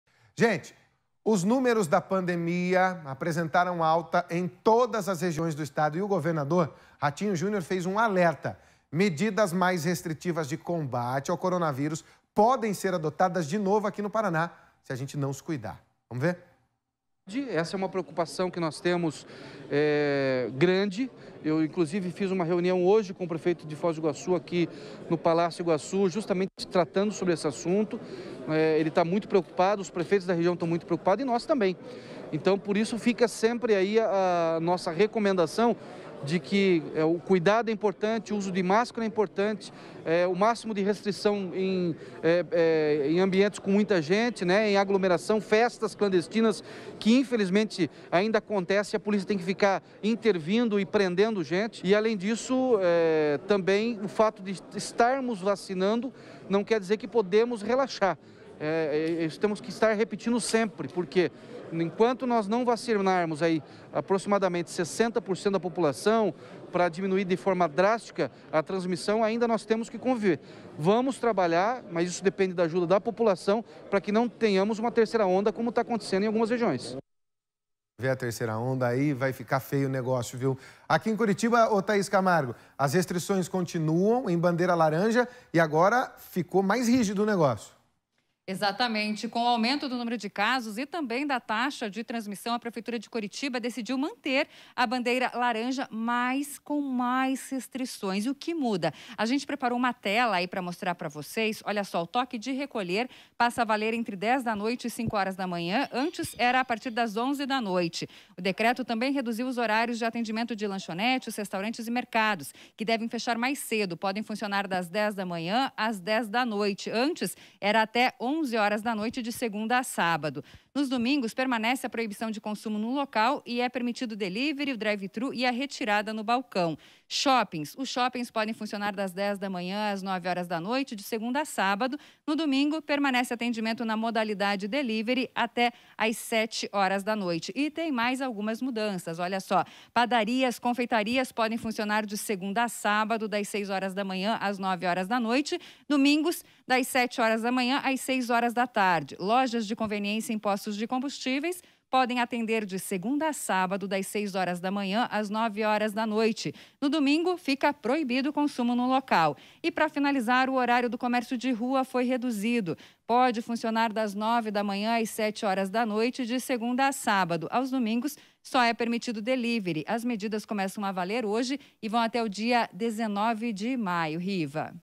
Depois de nova alta nos números medidas mais restritivas podem voltar
Os números da pandemia apresentaram alta em todas as regiões do estado. E o governador do estado fez um alerta. Medidas mais restritivas de combate ao coronavírus podem ser adotadas no Paraná. Confira todos os detalhes na reportagem!